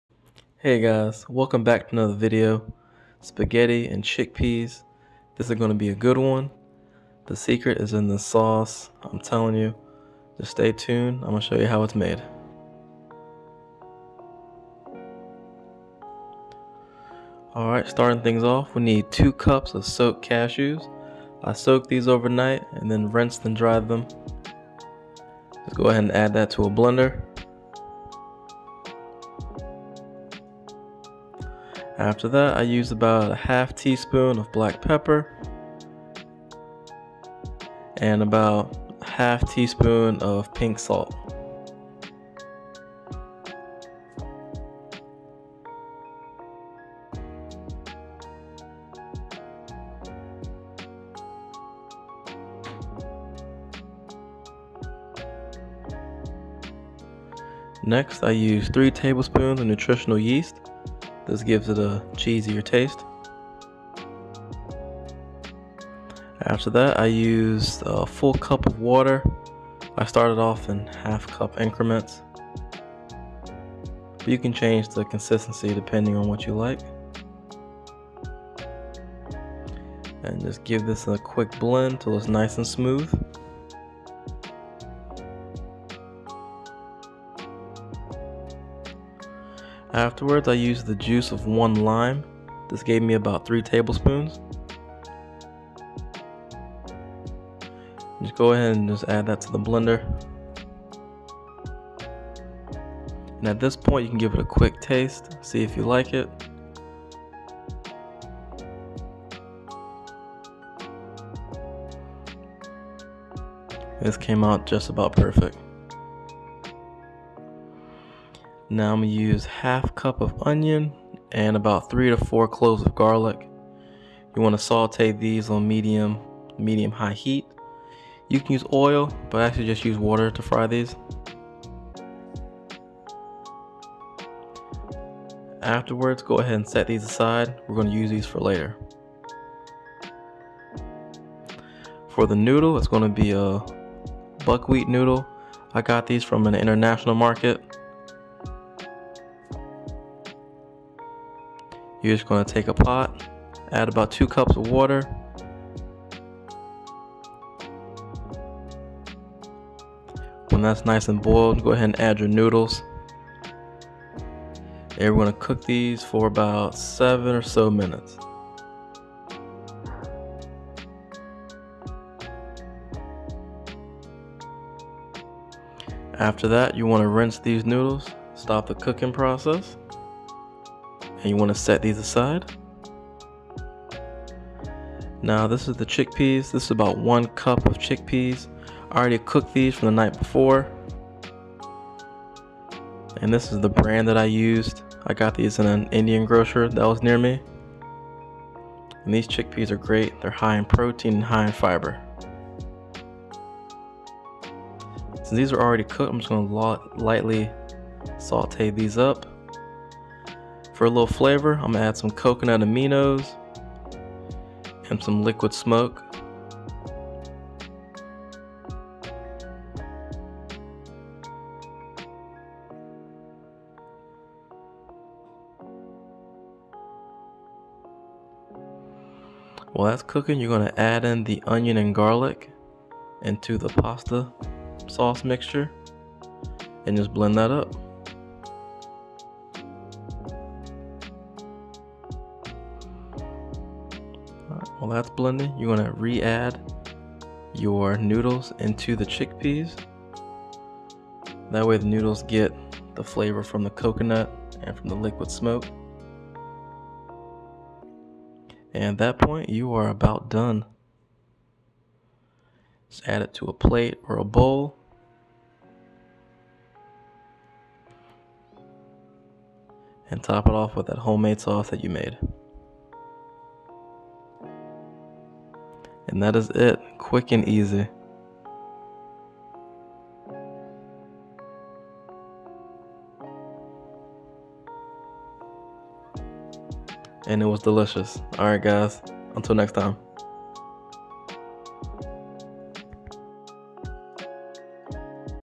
Spaghetti & Chickpeas!! | Dairy Free
What's creamy, savory, and affordable?? This recipe! Try this recipe out and let me know what you think.